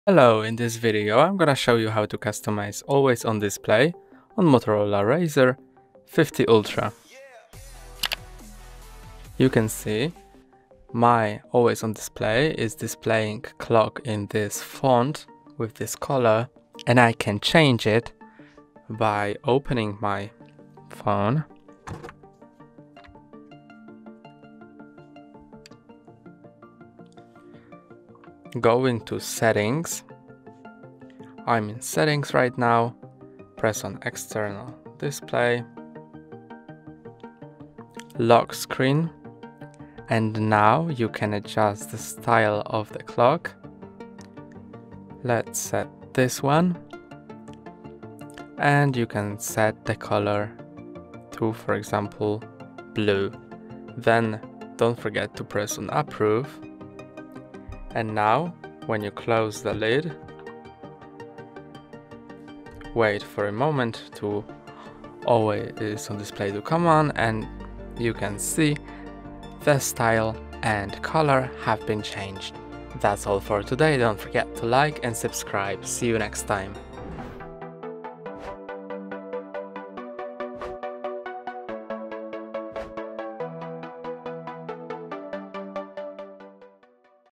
Motorola Razr 50 Ultra - Customize Always On Display - Personalize Your Screen
In this tutorial, we'll guide you on how to customize the Always On Display (AOD) on your Motorola Razr 50 Ultra. The Always On Display feature allows you to keep essential information visible, such as the time, date, and notifications, even when your screen is off. Customizing it to fit your style and needs enhances both functionality and aesthetics.

We'll walk you through the steps to personalize your Always On Display, from choosing different clock styles to adjusting what information appears. Follow our guide to make your Always On Display uniquely yours!

How to access Always On Display settings on the Motorola Razr 50 Ultra
How to select and customize different clock styles for your AOD
How to adjust which notifications and information are displayed
Tips for optimizing Always On Display settings for battery efficiency
How to activate and deactivate Always On Display as needed